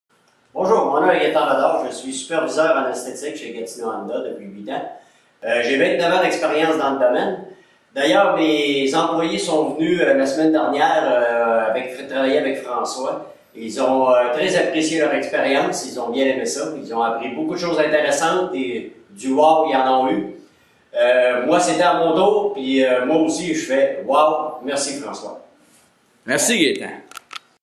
Gaétan Bédard - Gatineau Honda - CFEAQ - Juin 2012.MP4
CFEAQ - Centre de Formation Esthétique Automobile du Québec - Formation Technique